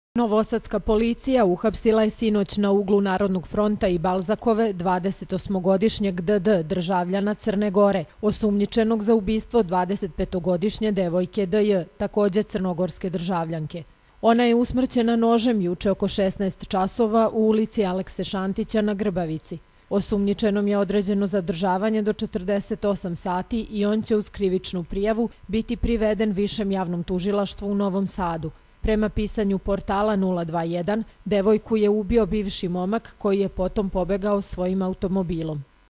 TV KANAL 9, NOVI SAD: UHAPŠEN ZBOG UBISTVA DEVOJKE
Novosadska policija uhapsila je dvadesetosmogodišnjeg državljanina Crne Gore zbog sumnje da je juče na Grbavici ubio devojku, takođe državljanku Crne Gore.